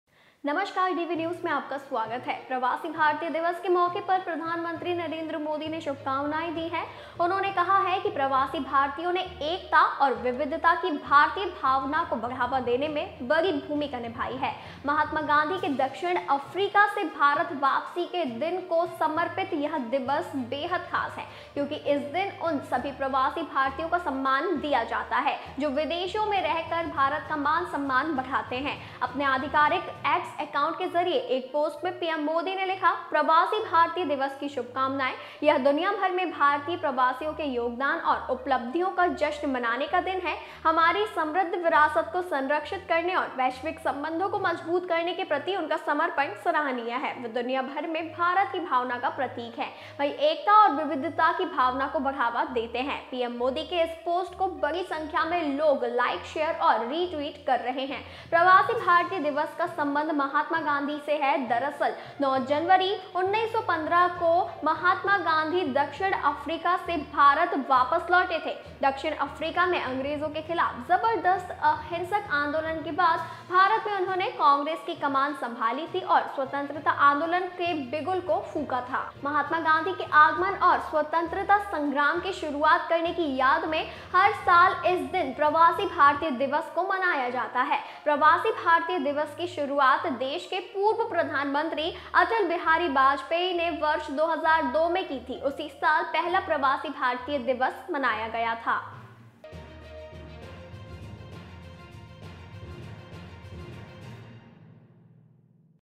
Pravasi Bharatiya Diwas: "NRI दुनिया भर में भारत की भावना के प्रतीक" PM Modi Wishes On NRI Day
PM Modi Wishes On NRI Day: Prime Minister Narendra Modi has extended best wishes on the occasion of NRI Day. He has said that NRIs have played a big role in promoting the Indian spirit of unity and diversity. This day is very special, dedicated to the day of Mahatma Gandhi's return to India from South Africa (9 January 1915). Because on this day, all those NRIs are honored who are enhancing the honor of India by living abroad.